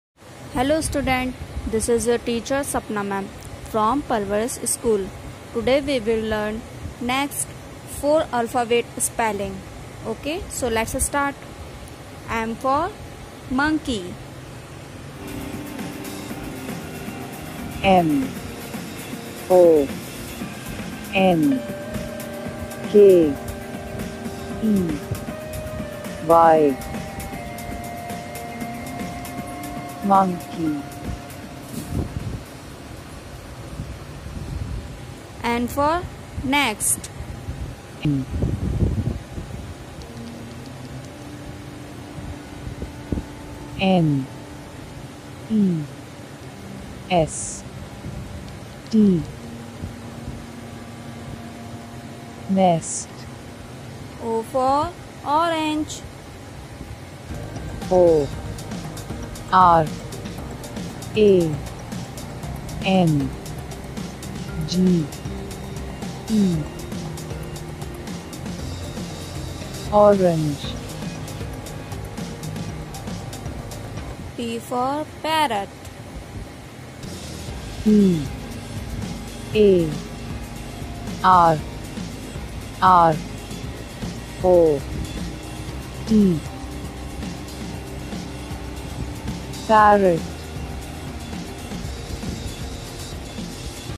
Class - Junior KG | English spellings of M N O P | PARVARISH SCHOOL, GWALIOR
This is the video for class - Junior KG
English spellings of M N O P
Regards
PARVARISH SCHOOL, GWALIOR